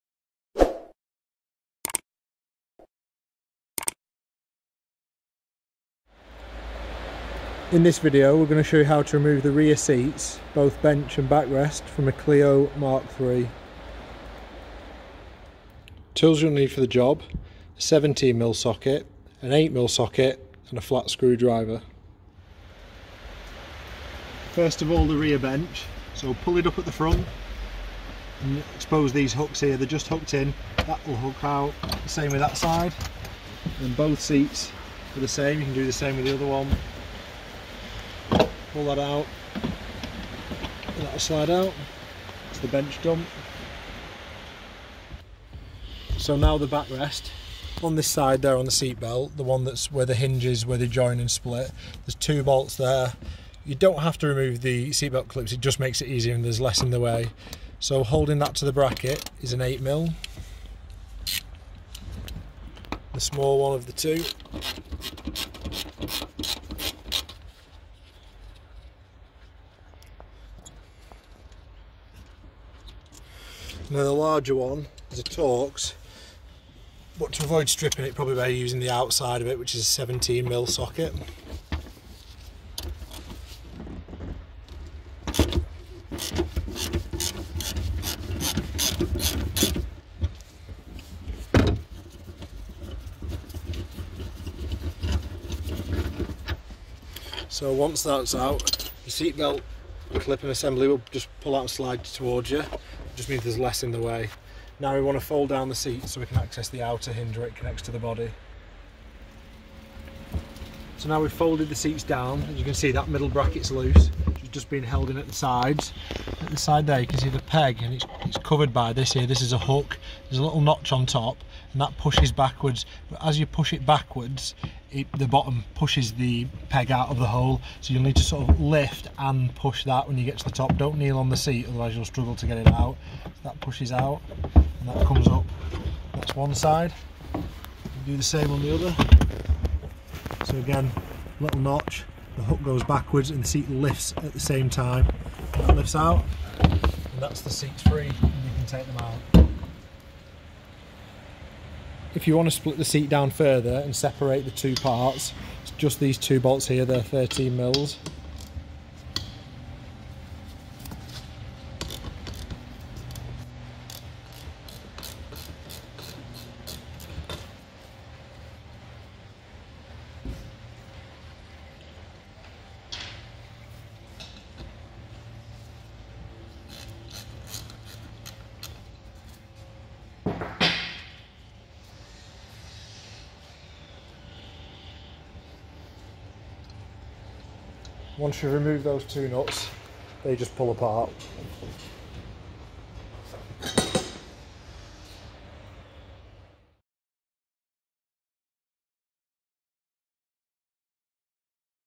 RENAULT CLIO 3 - How To Remove Rear Seats Bench & Backrest Removal 2006-2012
Guide on how to remove the rear seats from a Renault Clio III (Mk3), years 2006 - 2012.

Tools used:

-8mm Socket
-17mm Socket
-Flat Screwdriver